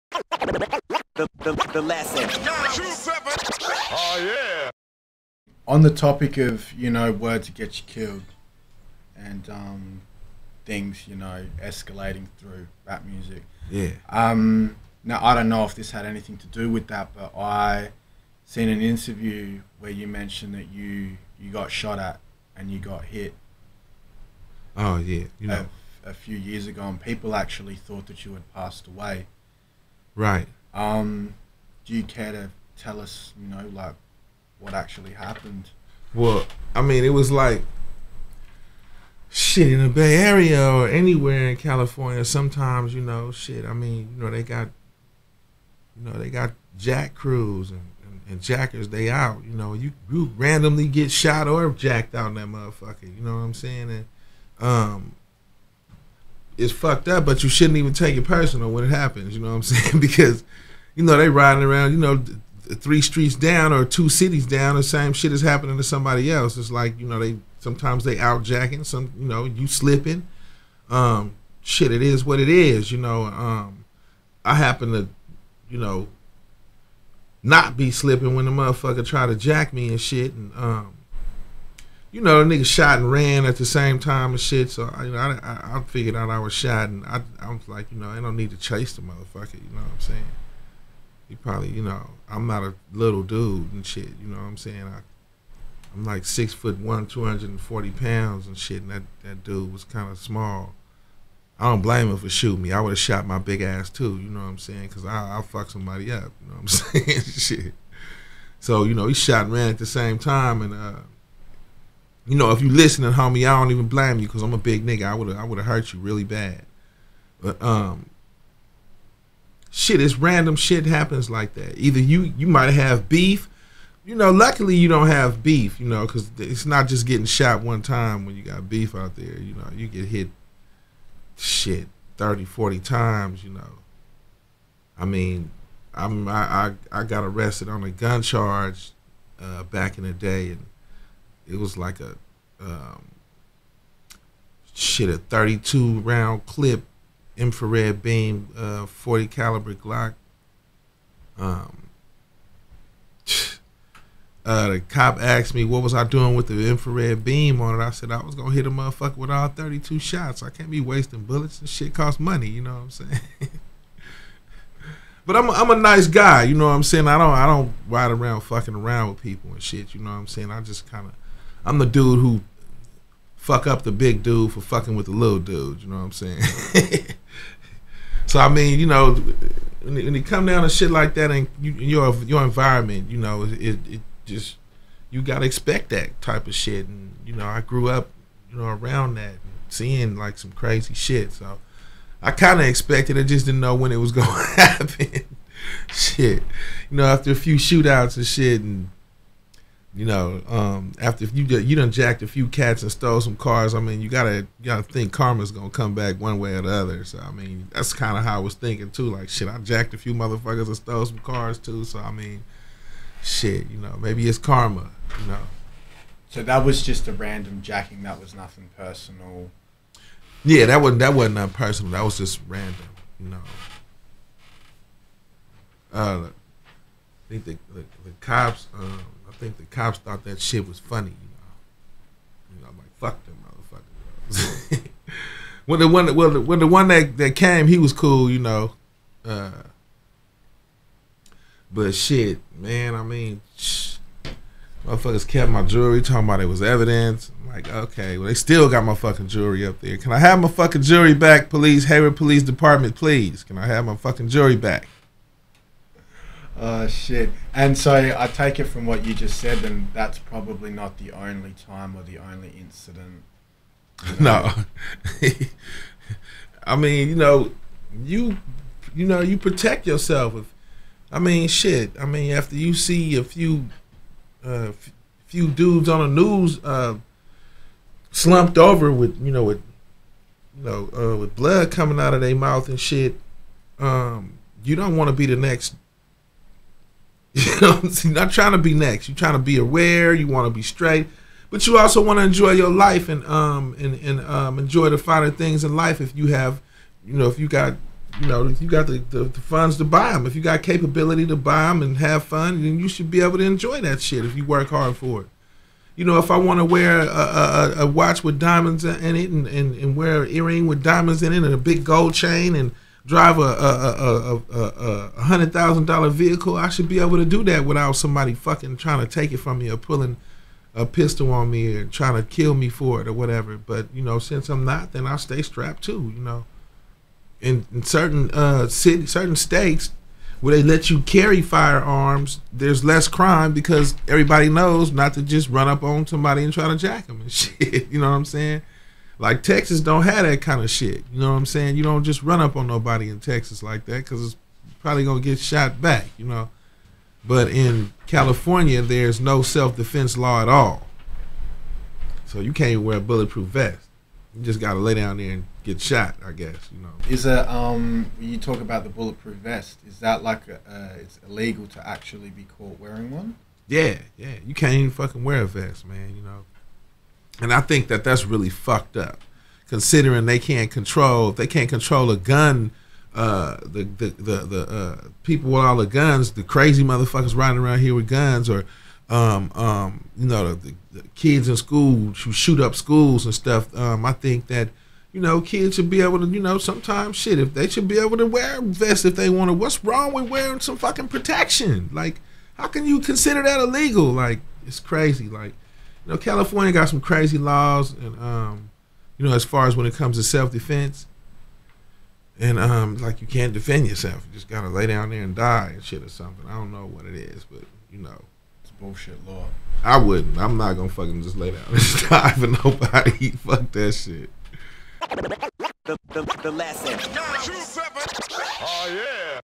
Spice 1 Talks About Getting Shot Back in 2007 “The Cops Thought That Sh*t Was Funny”
In 2007 Spice 1 was shot in an attempted jacking. He says the police thought it was funny and kept his jewelry for ‘evidence’ and never got it back. Discusses the lack of self defense laws in California and how it is illegal to be caught wearing a bullet proof vest.

Credits
Interview by Dj Sanchez
Filmed by PJ Gjonaj
Edited by Kyle Brand